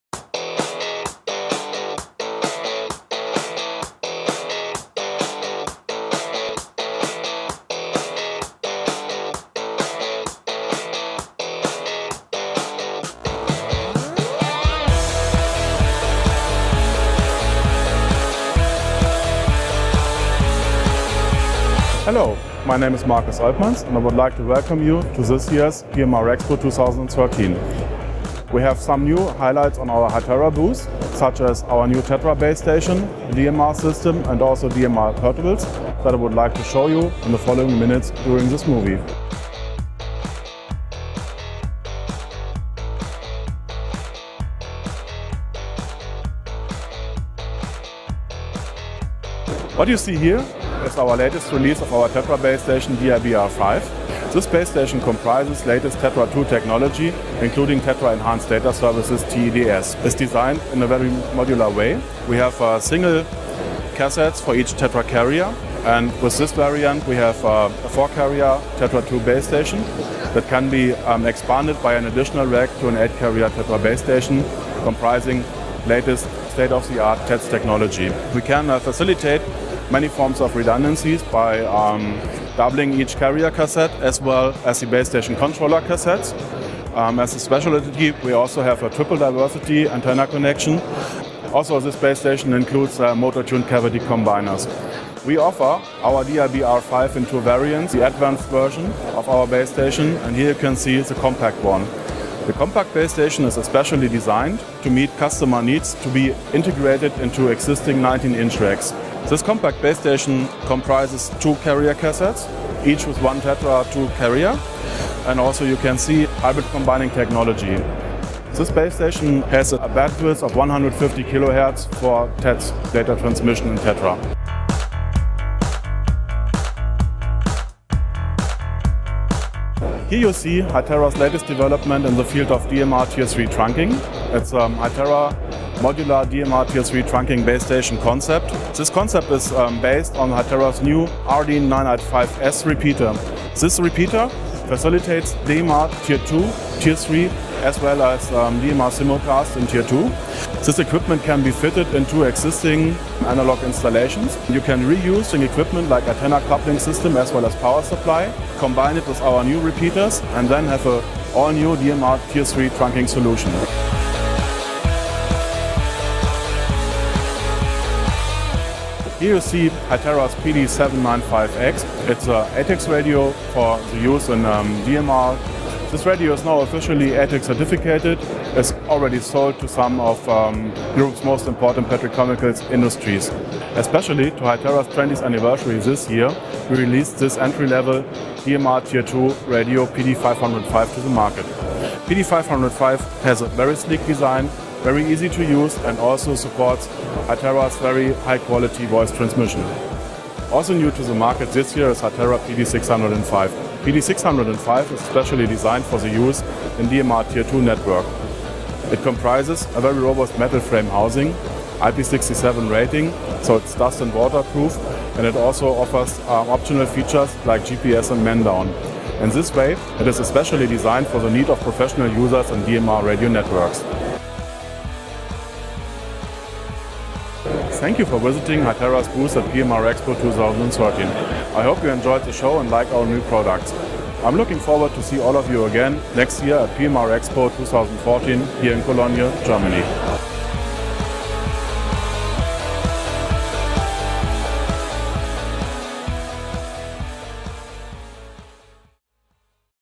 Hytera at PMRExpo 2013
Hytera showcases new entry level DMR portables at PMRExpo 2013. Compact PD50X and PD60X portables on show along with DMR ATEX radio, applications, base stations and dispatchers.